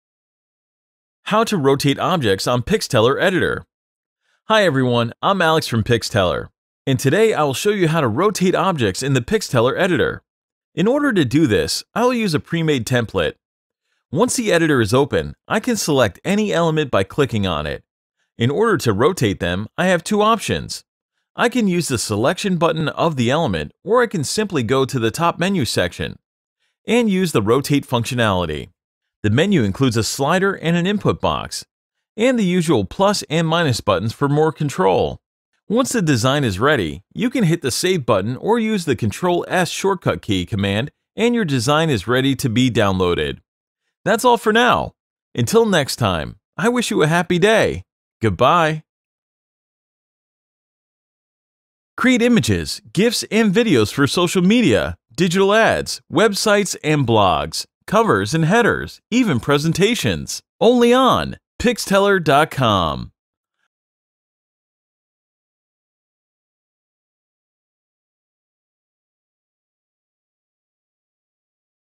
How to Rotate Objects in PixTeller Image Maker
Learn how to rotate objects in the PixTeller graphic maker in order to make your own images & animated GIFs in minutes.

Create your own astonishing graphics and animations with PixTeller's free easy-to-use, online design editor tools, and professional pre-made graphic templates.